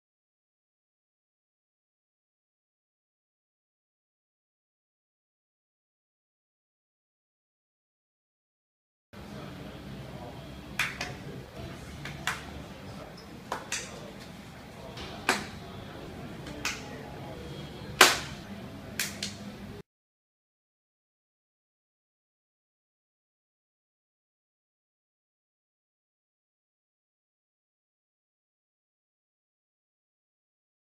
The Clap Virus Prank
I tricked a co-worker into thinking he had a computer virus called "The Clap". The Clap makes your cursor goes haywire until you to clap at the monitor. - There's no virus of course, I simply attached an extra wireless mouse to his computer when he went to the bathroom